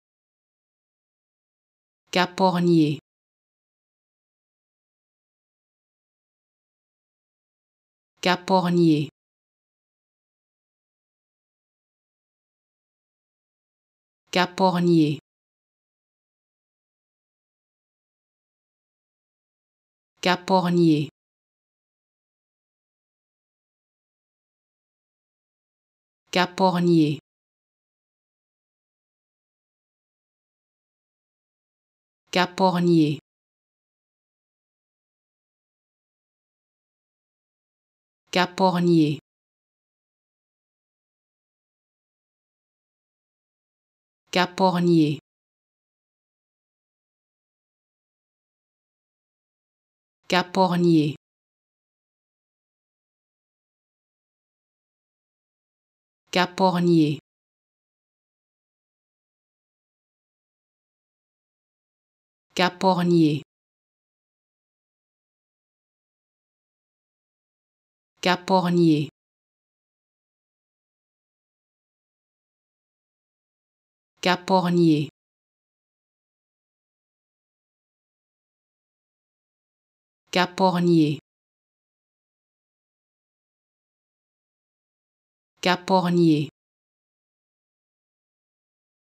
How to Pronounce ''Cap-hornier'' (Cape hornier) Correctly in French
Learn how to say and properly pronounce ''Cap-hornier'' in French with this free pronunciation tutorial.